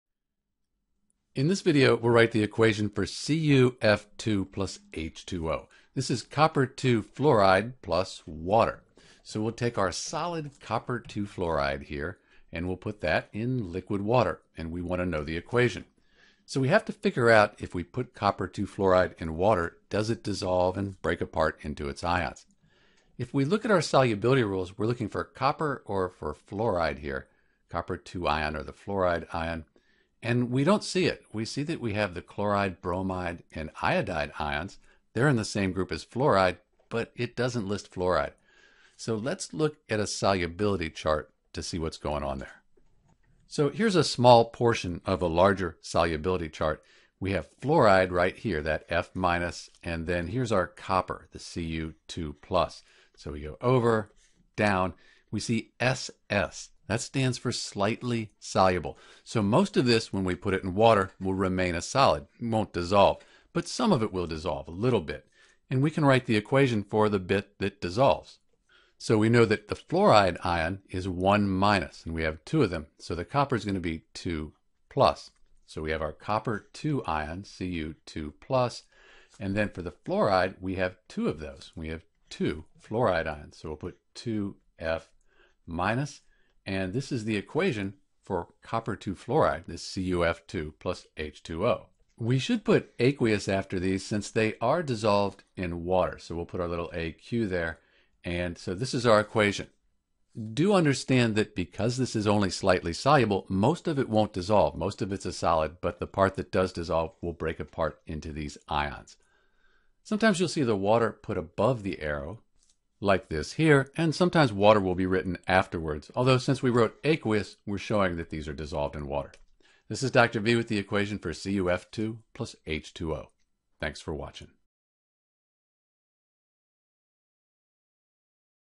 How to Write the Equation for CuF2 + H2O
In this video we will describe the equation CuF2 + H2O and write what happens when CuF2 is dissolved in water.  The answer is that it is slightly soluble in water. Although it is an ionic compound, it does not dissociate much into its ions in water, however a small amount does dissolve.

When this small amount of CuF2 is dissolved in H2O (water) it will dissociate (dissolve) into Cu 2+ and F- ions. To show that they are dissolved in water we can write (aq) after each. The (aq) shows that they are aqueous – dissolved in water.

The equation for CuF2,Copper (II) fluoride, and H2O sometimes isn’t considered a chemical reaction since it is easy to change the Cu 2+ and F- back to CuF2 (just let the H2O evaporate). At the same time, the CuF2 is a very different substance than Cu 2+ and F-.

Knowing the solubility rules can help you determine whether CuF2 will dissolve in water and dissociate into ions